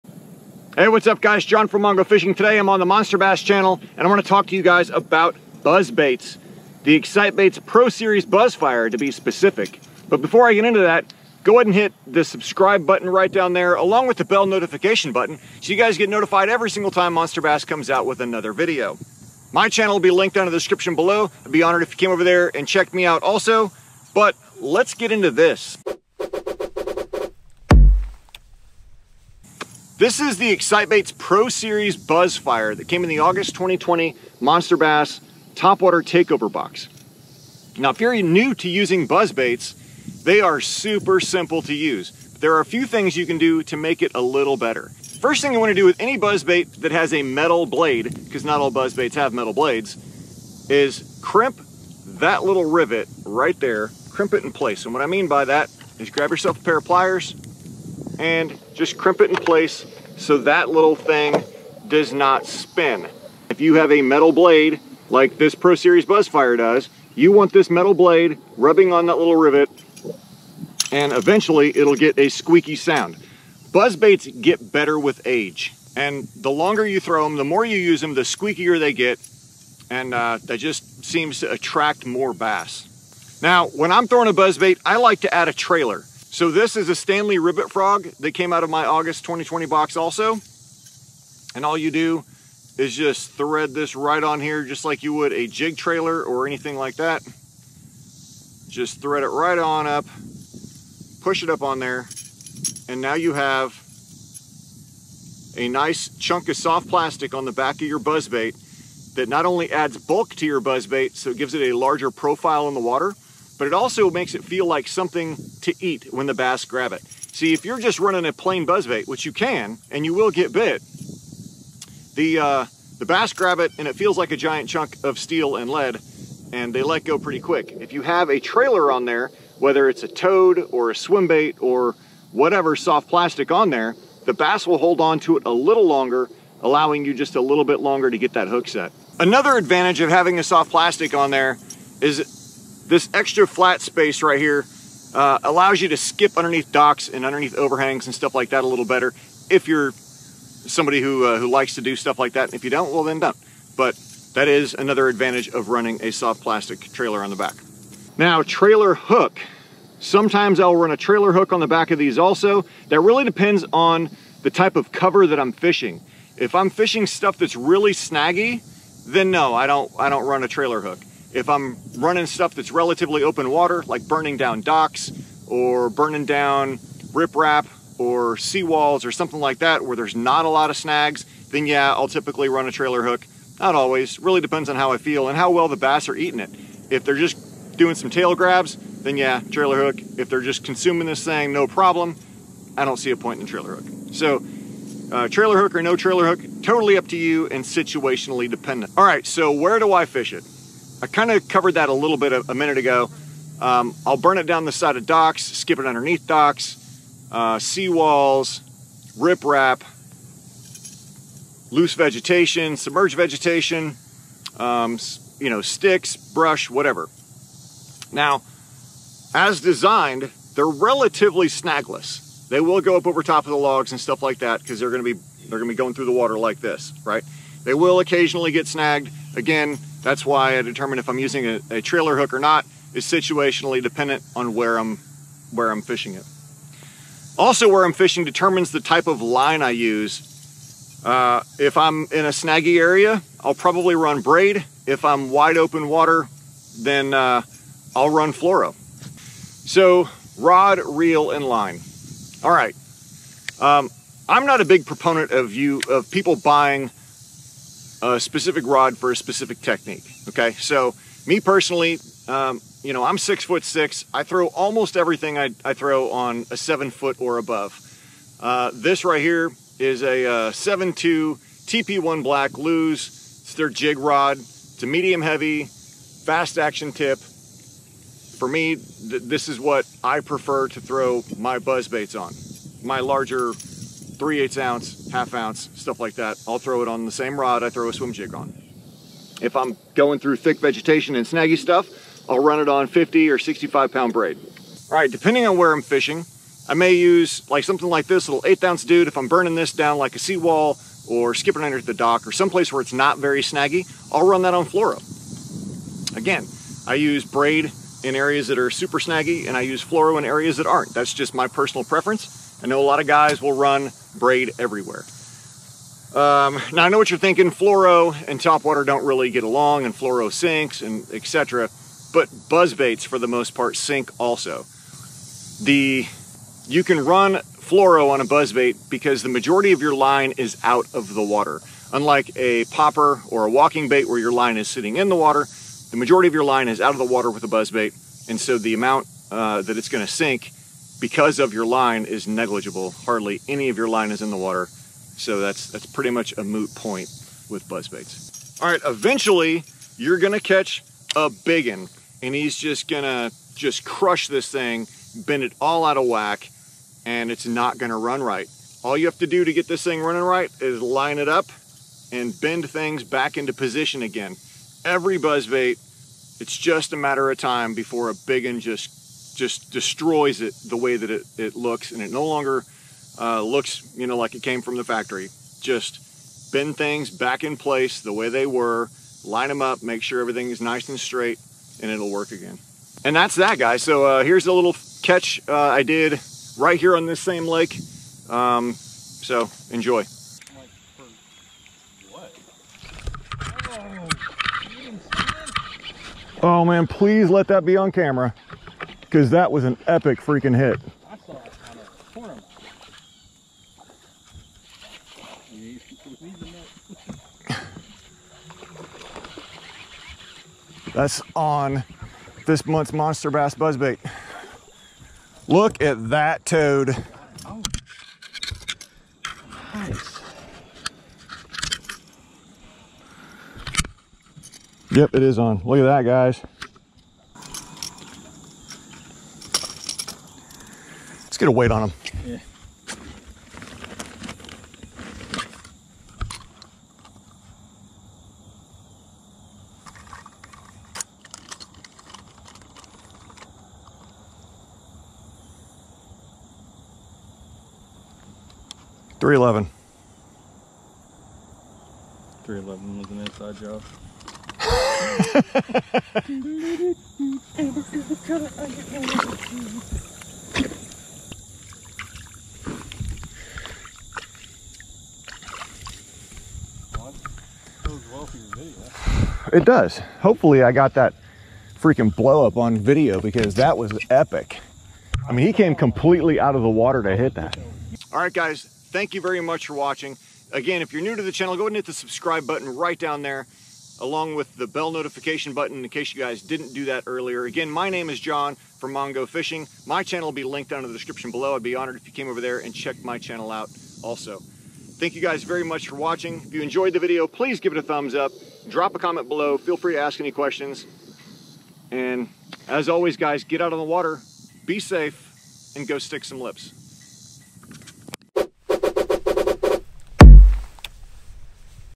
Buzzbait Fishing TIPS For TOPWATER Bass Fishing
Check out @MongoFishing's tips to land more fish on a buzzbait!  Buzzbait fishing can be one of the most fun and effective topwater fishing methods out there.  Take these bass fishing tips out on the water this week!